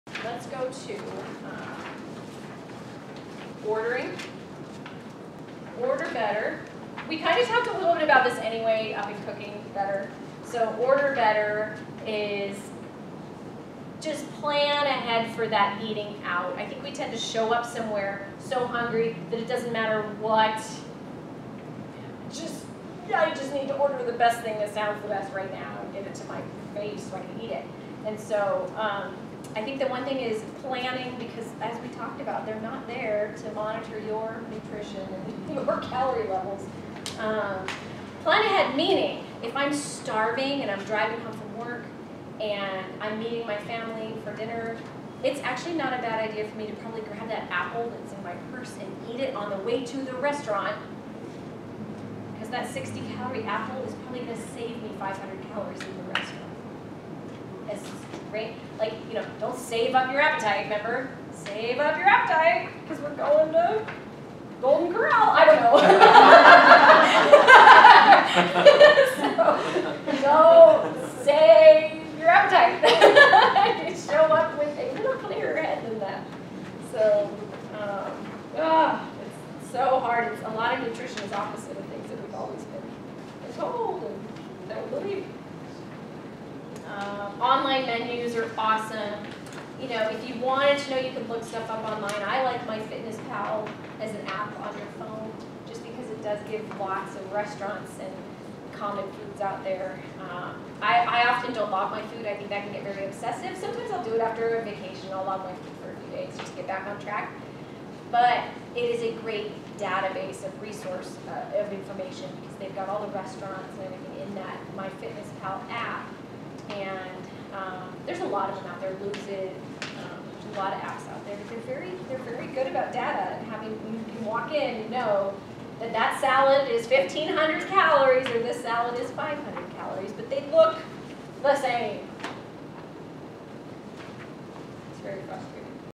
Nutrition in 8 Steps - Step 6 Order Better
Eating at a restaurant does not have to sabotage your plan.